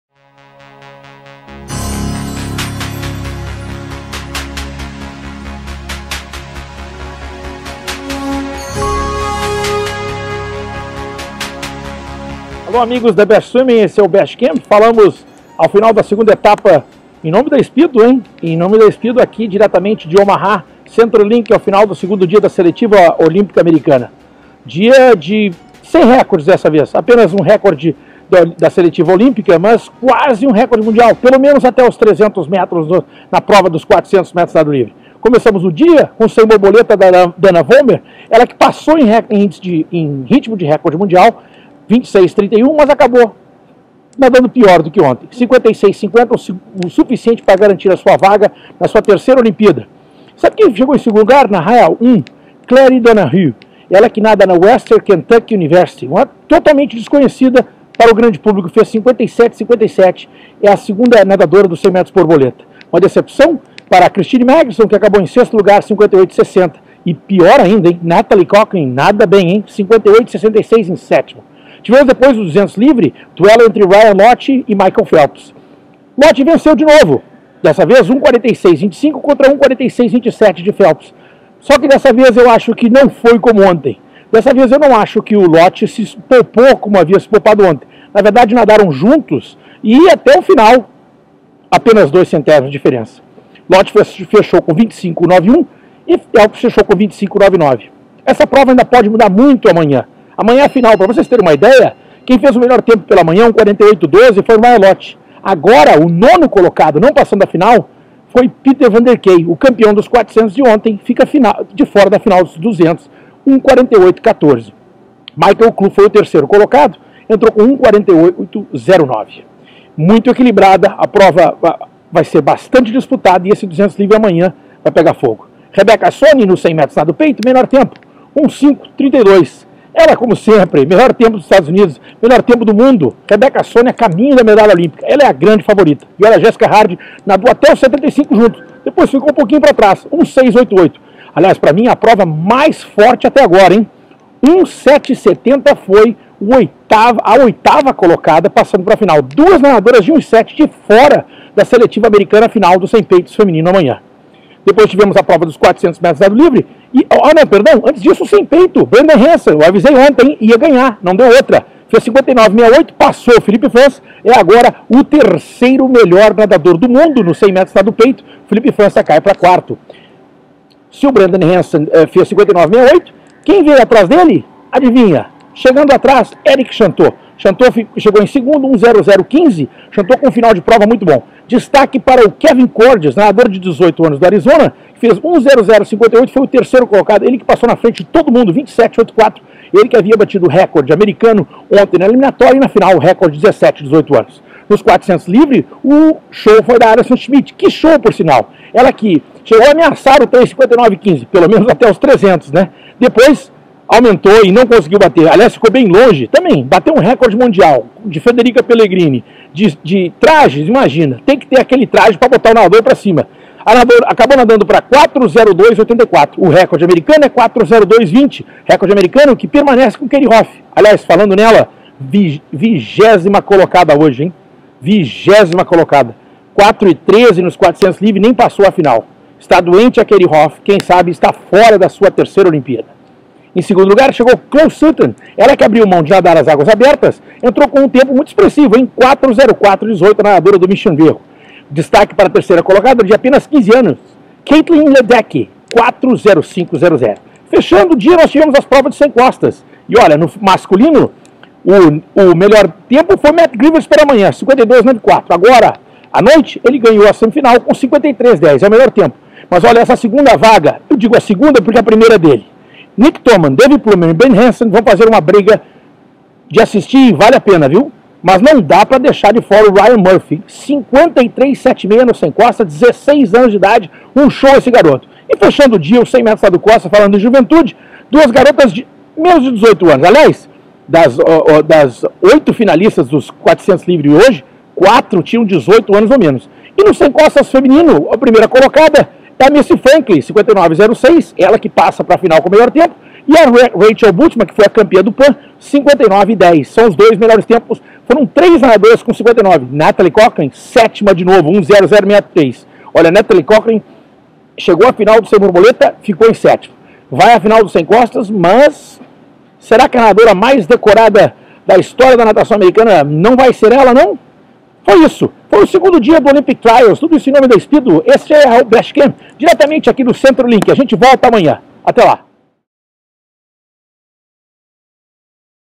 Best Cam U.S. Olympic Trials - dia 2
Cobertura da Best Swimming na Seletiva Olímpica Americana, que acontece de 25 de junho a 1o. de julho, na cidade de Omaha.